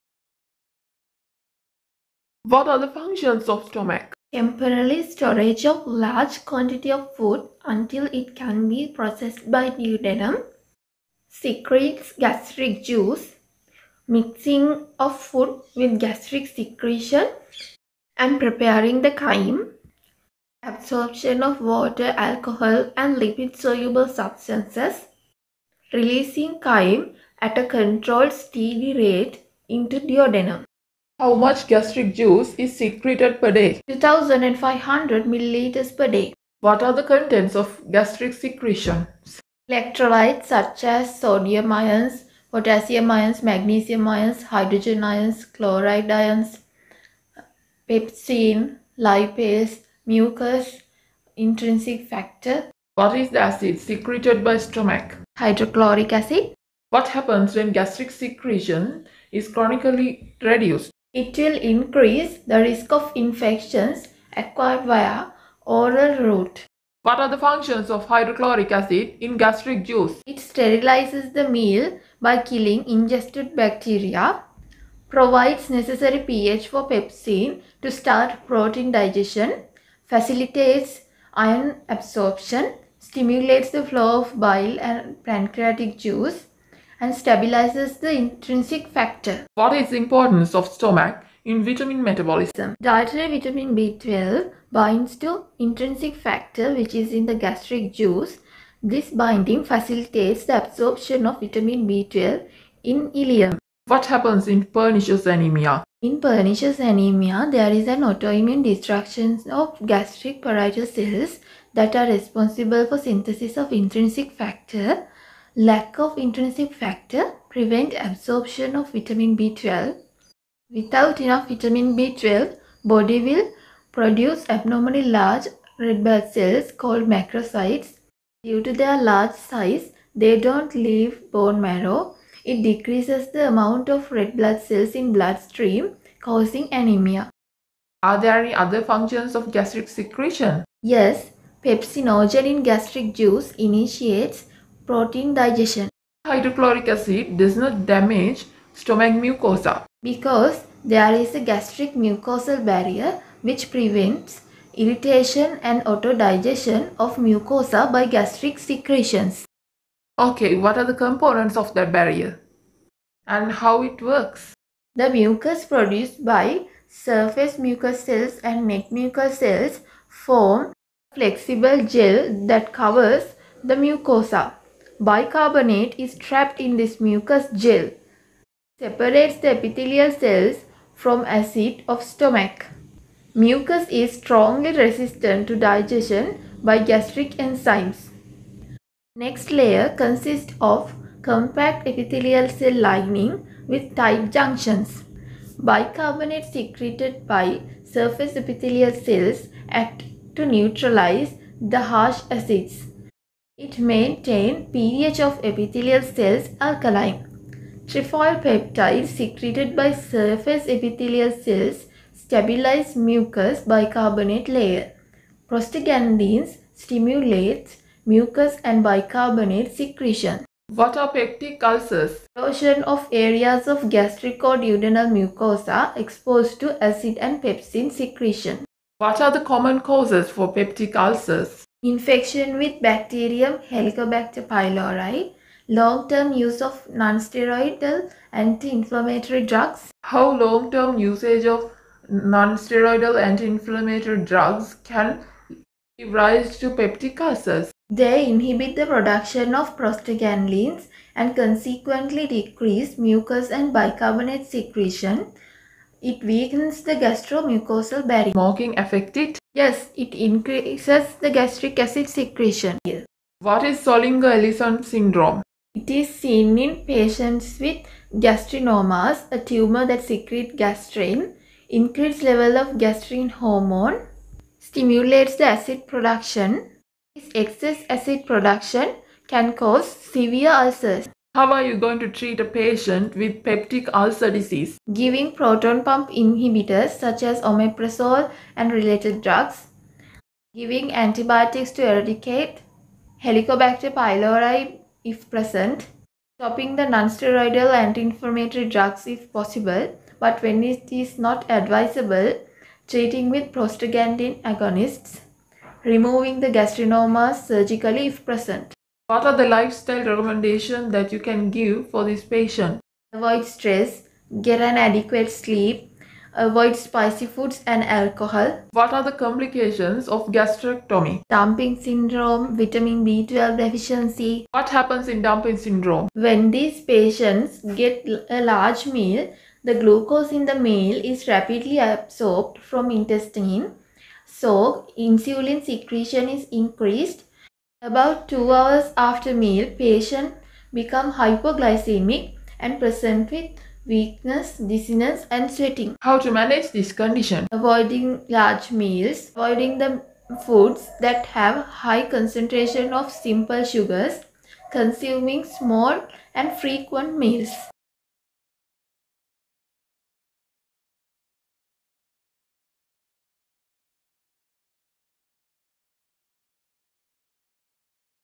Physiology viva questions | Gastrointestinal physiology viva | physiology viva questions and answers
This video is about Physiology viva questions | Gastrointestinal physiology viva | physiology viva questions and answers
This video includes  a demonstration of a viva examination of physiology. The main area related to this viva session is Gastrointestinal  physiology

Related topics -
stomach
gastric secretion
gastric mucosal barrier
peptic ulcer disease
gastrectomy